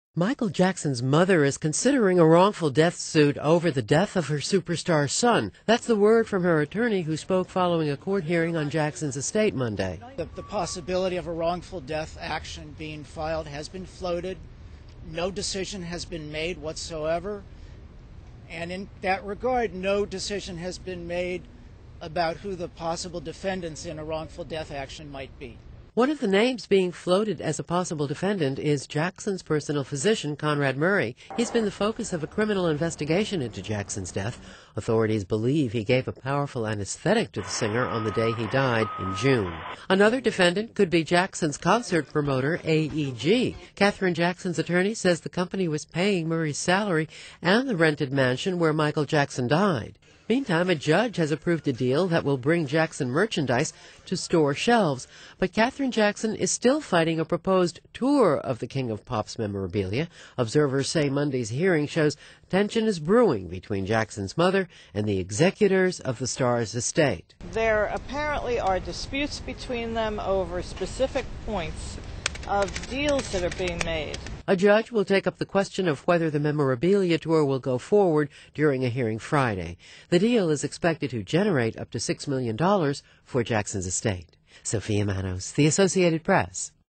An attorney for Michael Jackson's mother says she is considering a....
An attorney for Michael Jackson's mother says she is considering a wrongful death lawsuit because of the circumstances surrounding her son's demise, and that the singer's personal physician is a likely target. (Aug. 18)